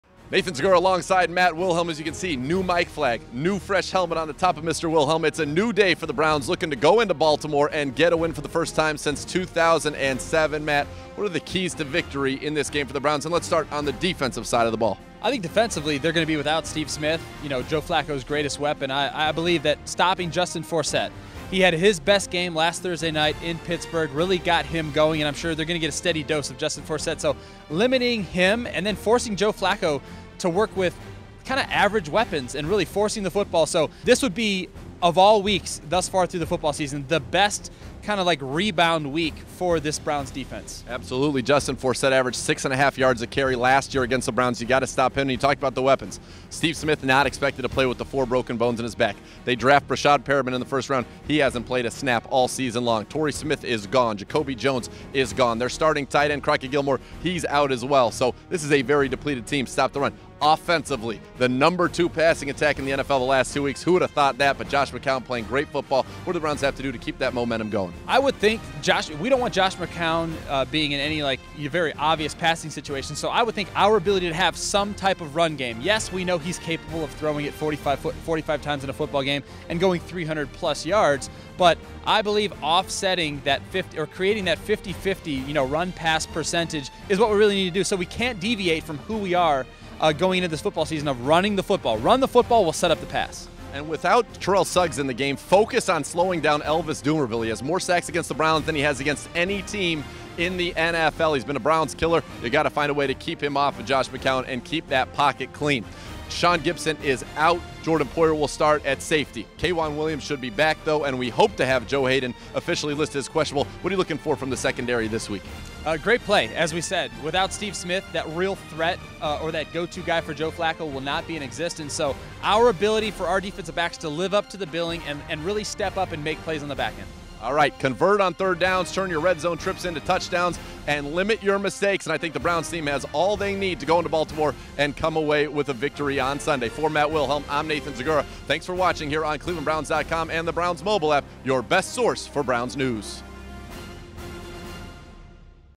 Gameday Report: Browns vs. Ravens
Nathan Zegura and Matt Wilhelm preview the game between the Browns and the Ravens.